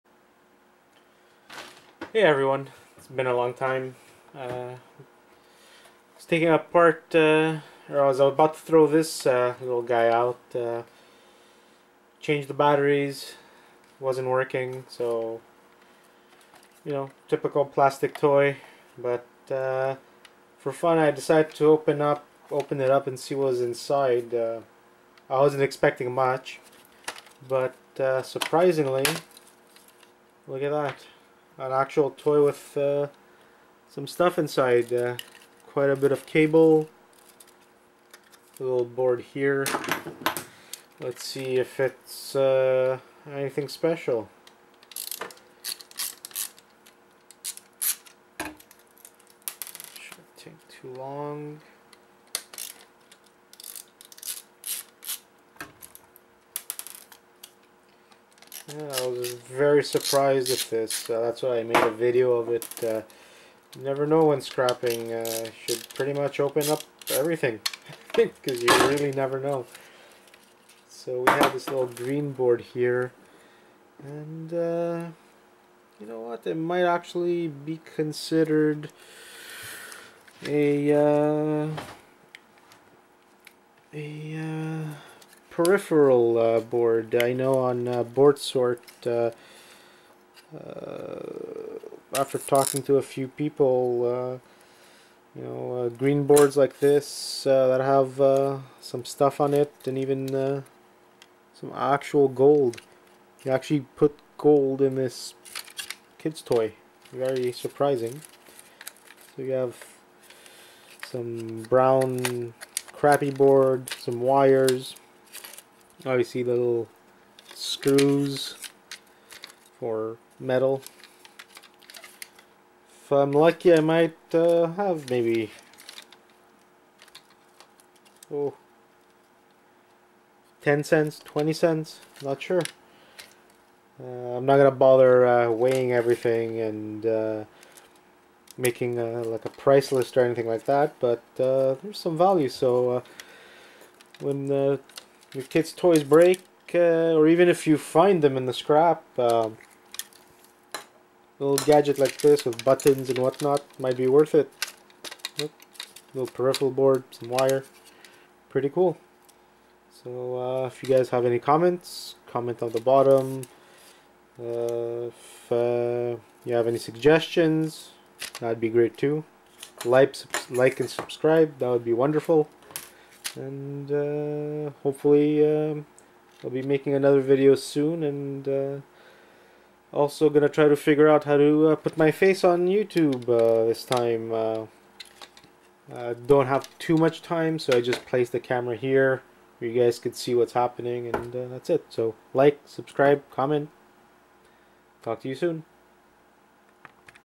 Scrapping Kids Toys. Is there Money in Those Plastic Things??? Wire? Copper? Aluminium? Boards?
So in this video, I decided to look at one of my kids' toys that just stopped working. I was about to throw it in the trash and said to my self. Let me open it up. So I did and this is the video of it.